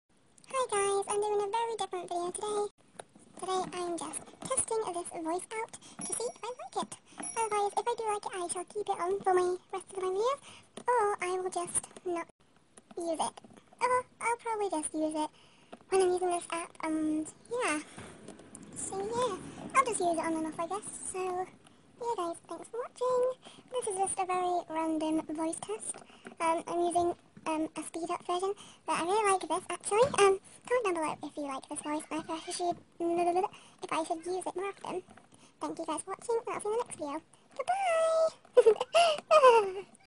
Voice Test! XD
Hi! Today I'm just testing speed-up version of my voice! Comment if you like it! Byaaaa!

My WattPad: BookManias
My animal jam username: Enchantedmagicglow
GO FIND ME XD